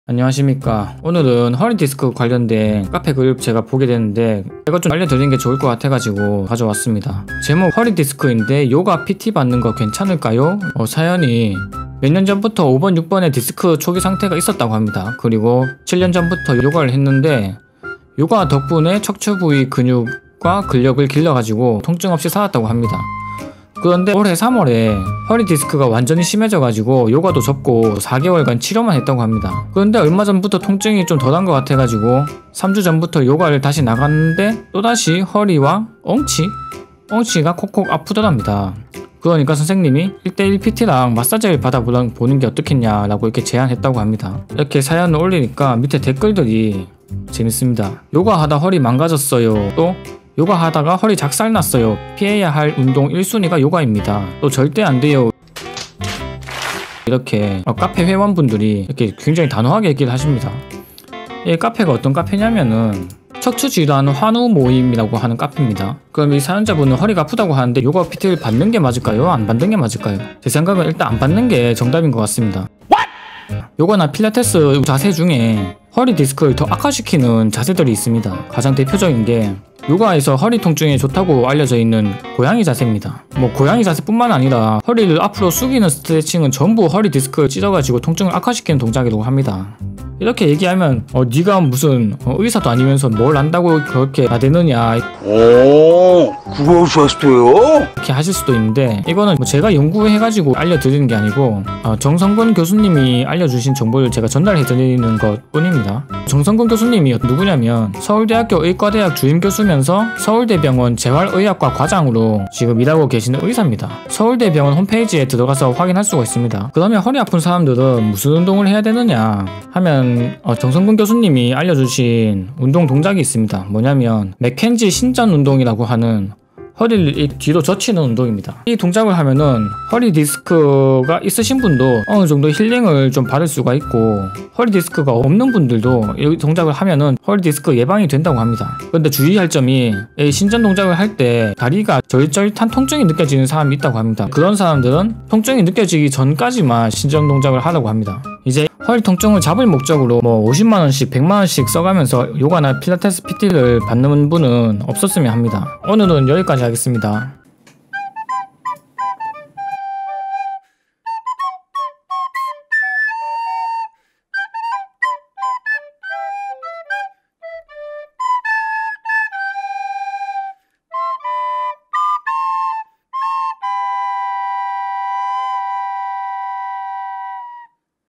요가, 필라테스 스트레칭이 허리디스크에 미치는 영향 (feat. 맥켄지)
오늘은 허리디스크있는 분이 요가나 필라테스를 만나면 어떻게 되는지에 대한 정보를 담에 영상을 만들었습니다.

 저도 한때 허리가 아팠던 사람으로써 허리 아프신 분들에게 도움이 되고자 해서 만들었는데 재밌게 봐주셨으면 좋겠네요.

책 - 백년운동